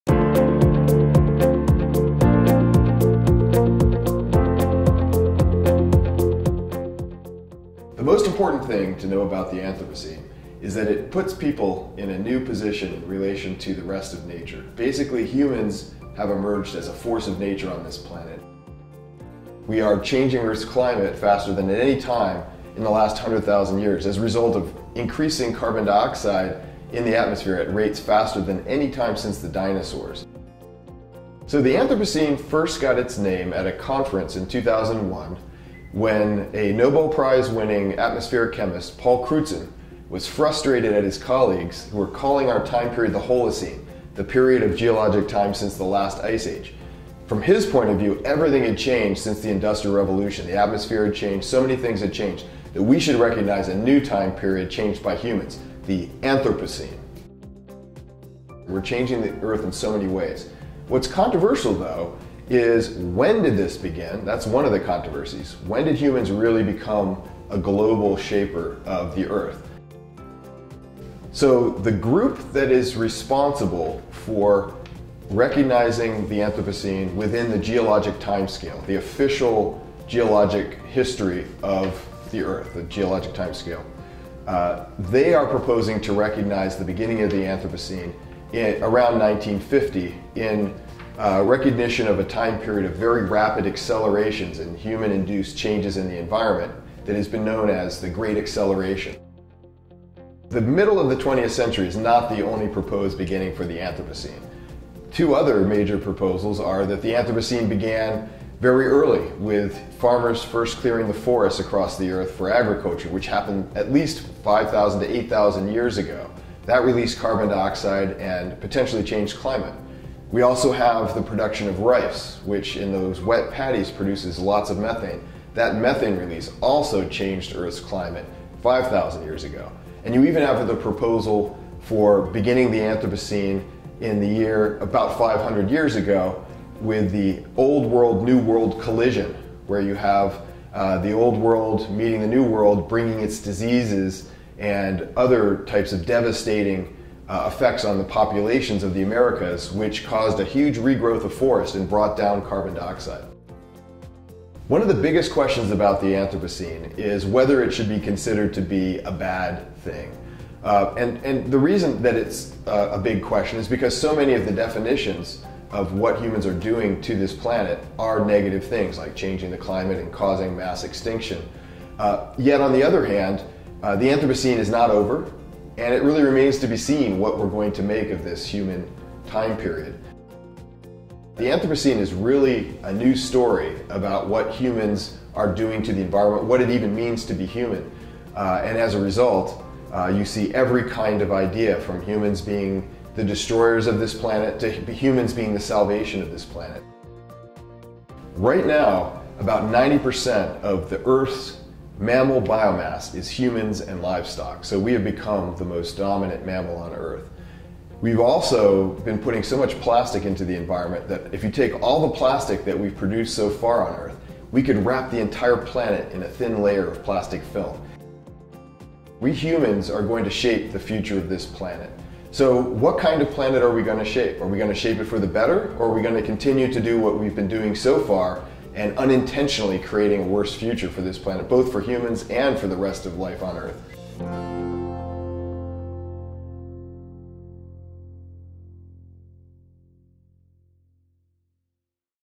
Anthropocene: A Very Short Introduction | Erle C. Ellis
Erle C. Ellis, author of, "Anthropocene: A Very Short Introduction", gives his top 10 things you should know about the Anthropocene which humanity has left its mark on the Earth, from pollution and the use of fossil fuels to mass extinctions.